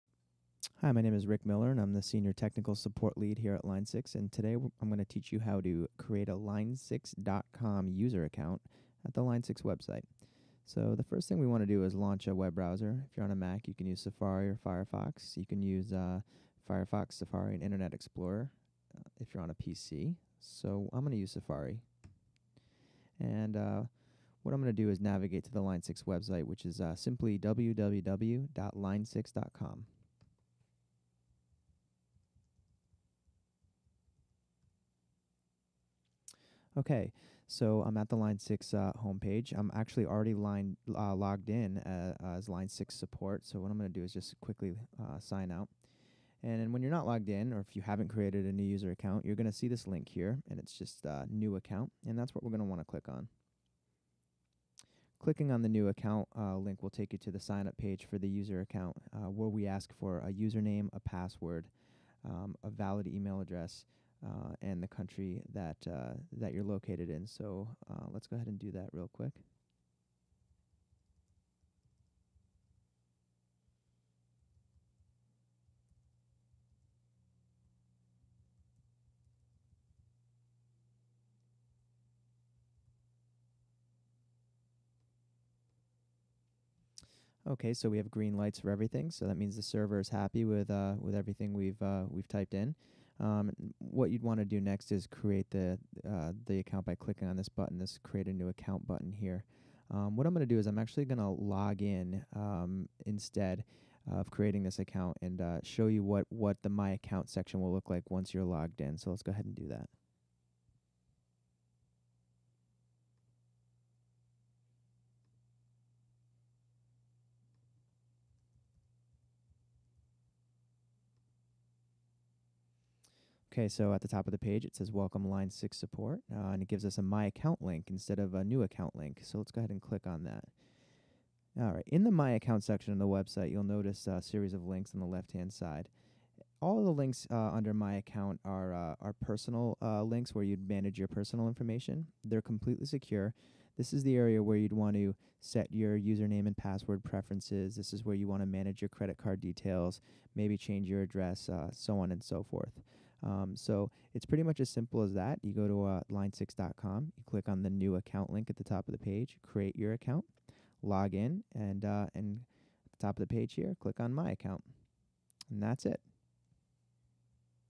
Creating a Line 6 User Account
How to correctly create a Line 6 user account on the Line 6 website